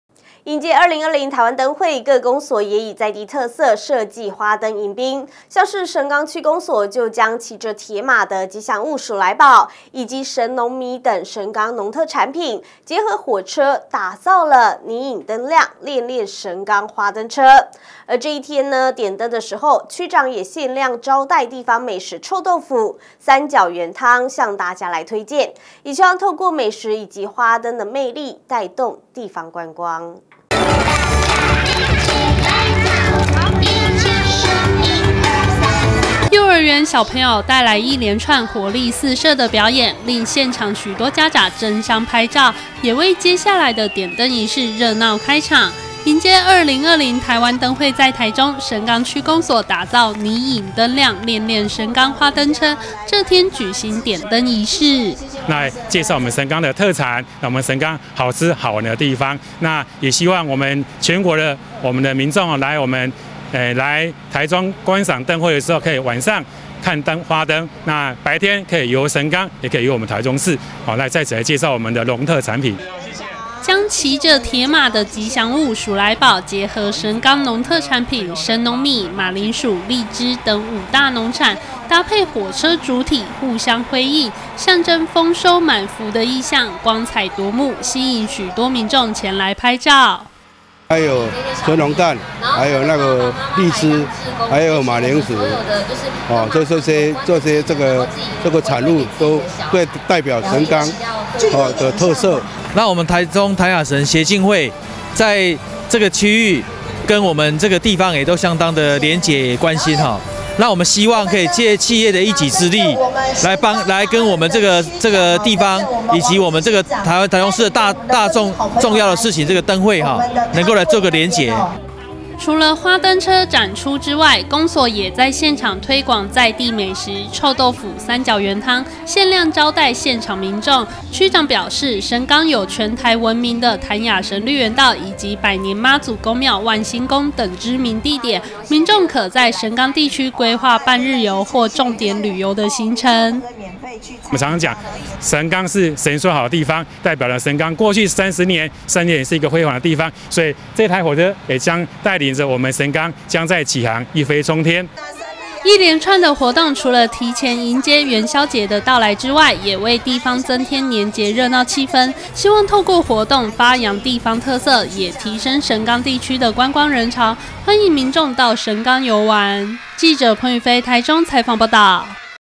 大台中新聞-2020台灣燈會在台中 薯來寶帶你遊神岡
迎接2020台灣燈會在台中，神岡區公所將騎著鐵馬的吉祥物「薯來寶」以及神農米、馬鈴薯等神岡農特產品，結合火車，打造「霓影燈亮、戀戀神岡」花燈車，這天舉辦點燈儀式，而除了宣傳神岡在地特色外，區長也在現場推廣臭豆腐、三角圓湯，限量招待現場民眾及貴賓們，透過活動，發揚地方特色，也希望提升神岡地區的觀光人潮，邀請民眾到神岡遊玩。
「以上新聞由top NEWS APP收看」